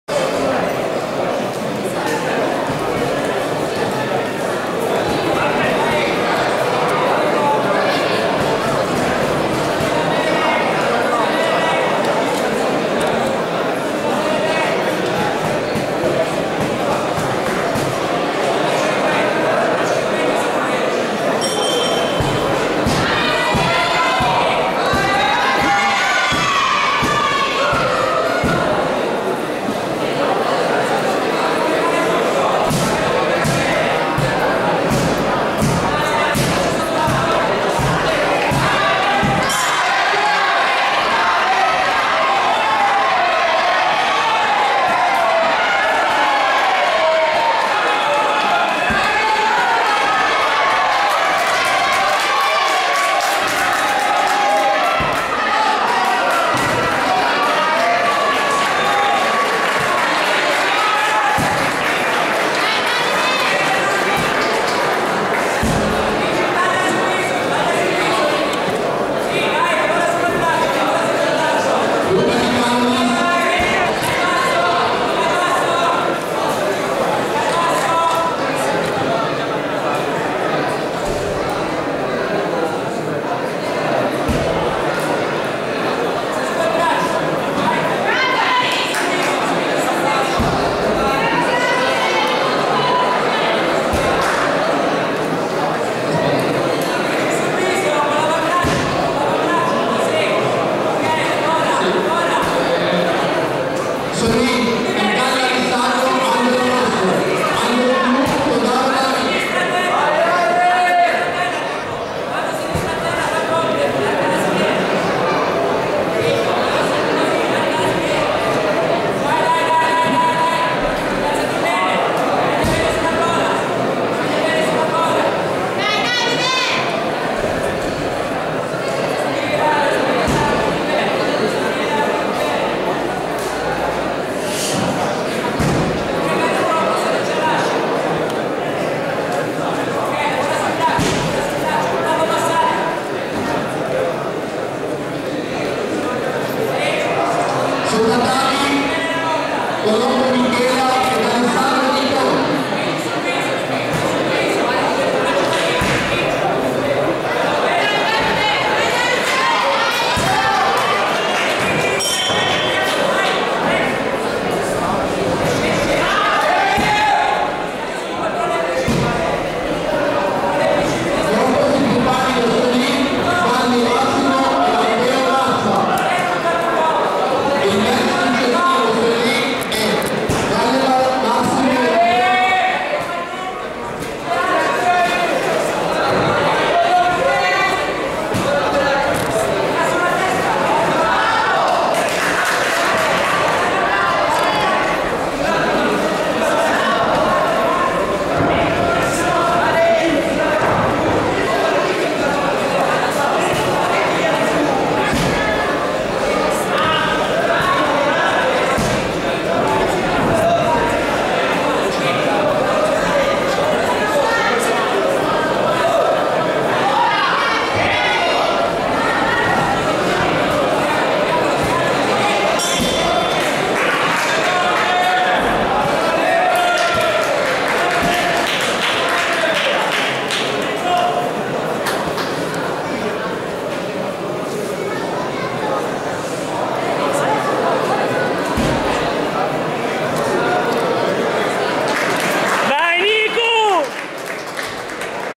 Reactive Club - Clopot Nicu - Grappling - TKC - 18/10/15
Gli atleti della Reactive Club si esibiscono al galà di sport da combattimento il 18/10/15